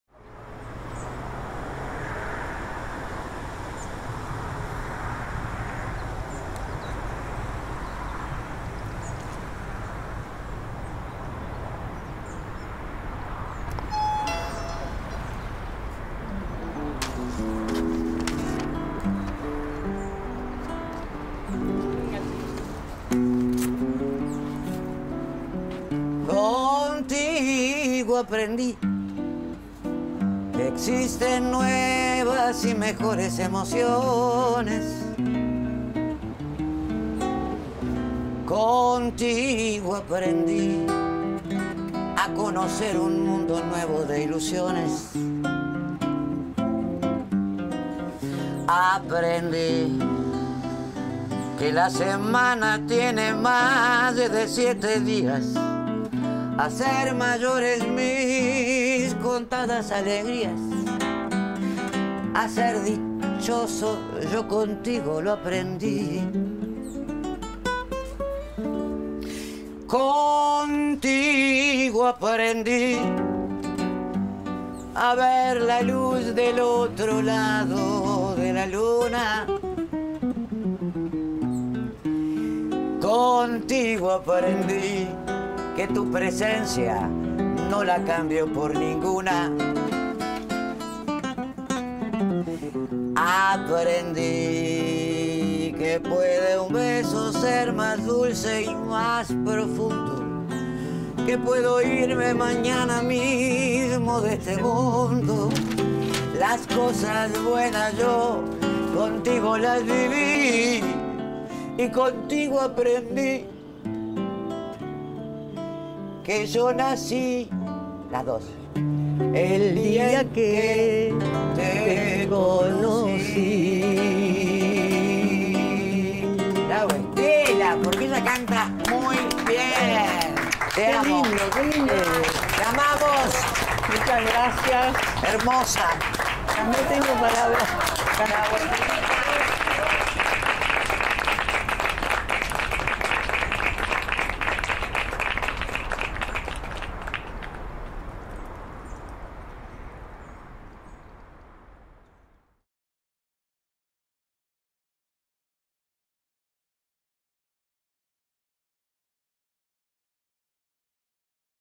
Serenatas Memorables: Estela de Carlotto
A 45 años del último golpe cívico-militar, junto al Ministerio de Cultura de la Nación, homenajeamos a nuestras mujeres luchadoras a través de este ciclo llamado SerenatasMemorables. Te invitamos a ver la segunda de ellas: Adriana Varela le canta a Estela de Carlotto.